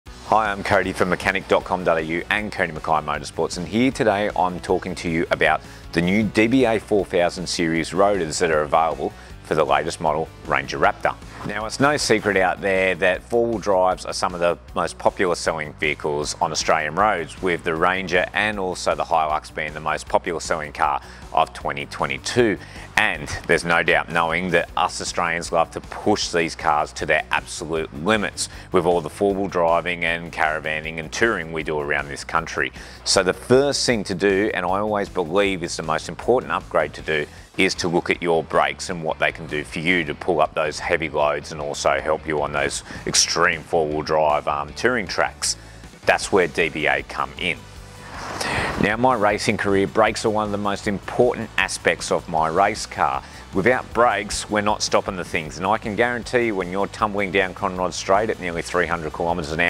PRODUCT SHOWCASE: DBA 4000 Series Brake Rotors for the latest model Ford Ranger Raptor
Join Cody from Mechanic.com.au as he takes an in-depth look at the latest 4000 Series Rotors from DBA for the Ford Ranger Raptor. Part Number: DBA43162S